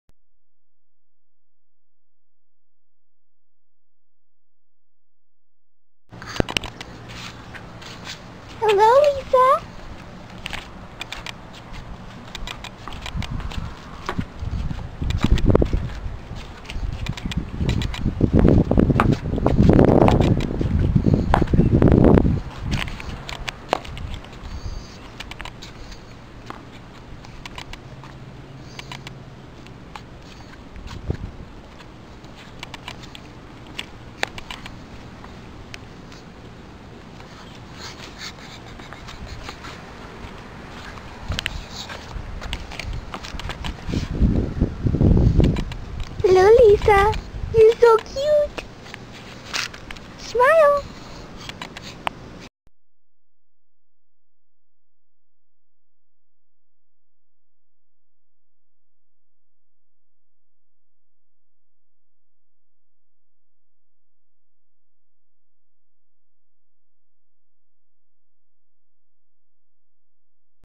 Princess Pug
Princess Pug Lisa Being pushed in her Princess Pink Stroller! This is our special guest, Lisa, by the way!